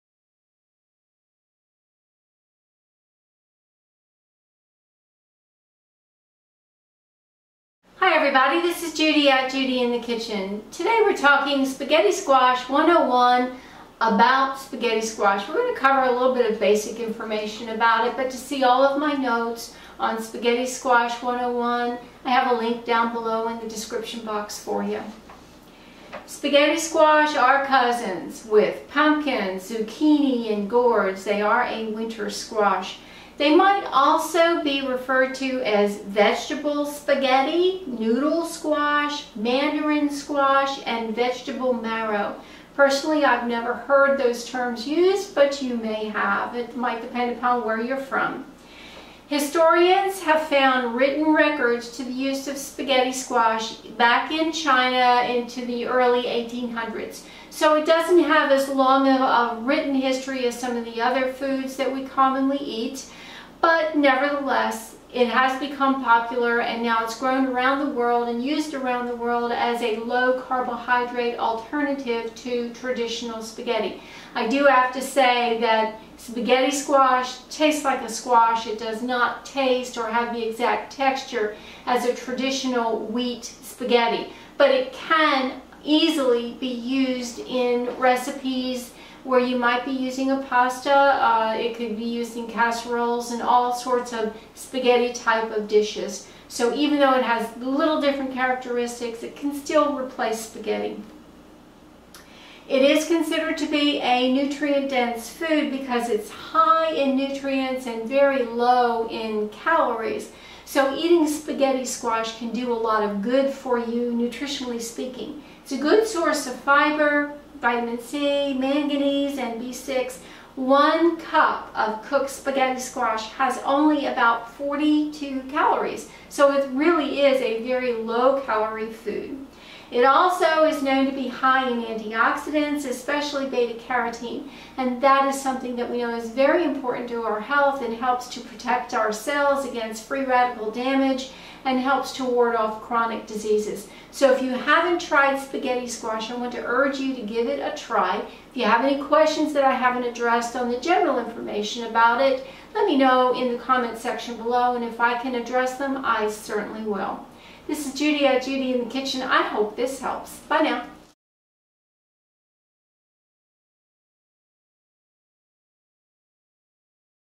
Spaghetti Squash 101 - About Spaghetti Squash
In this video, I cover the basics about spaghetti squash...what it is, how it can be used, and nutrition information.